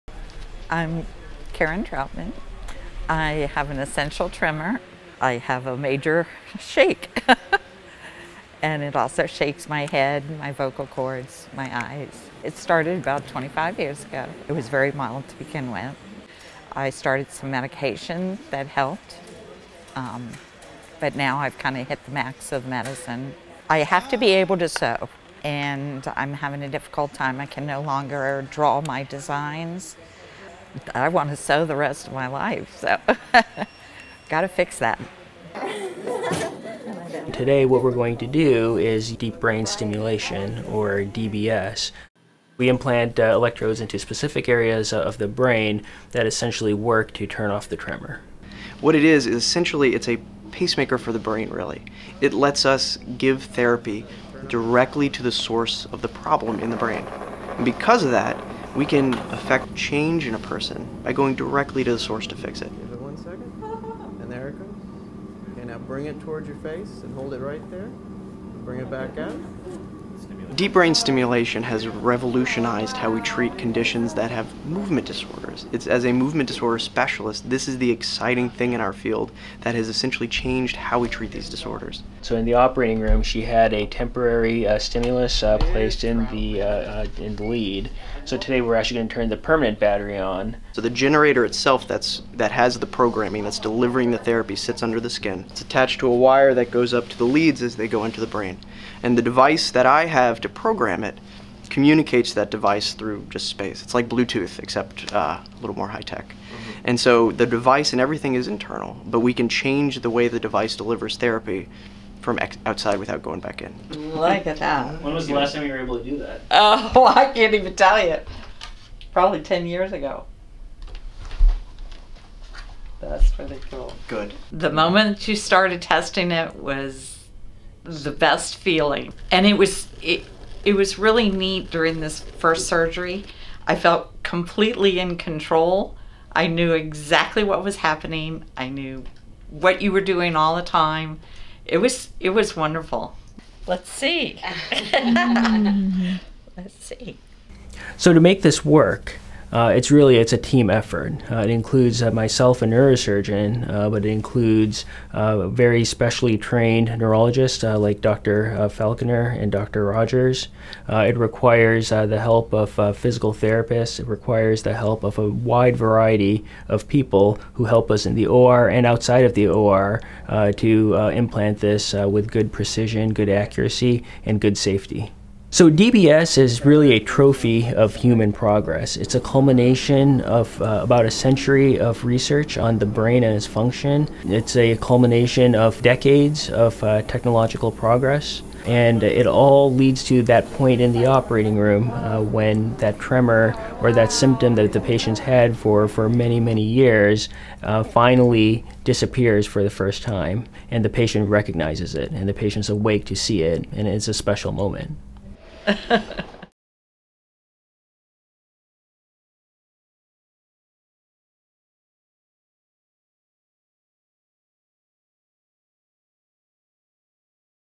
Karen Troutman Shares How Deep Brain Stimulation Treated an Essential Tremor
Karen Troutman has had an essential tremor for more than 20 years that has made it difficult to write and sew, which is her passion. Dr. Mahesh Shenai, MD, Neurosurgeon, and Dr. Drew Falconer, Neurologist, explain the Deep Brain Stimulation procedure and how it is used to treat an essential tremor.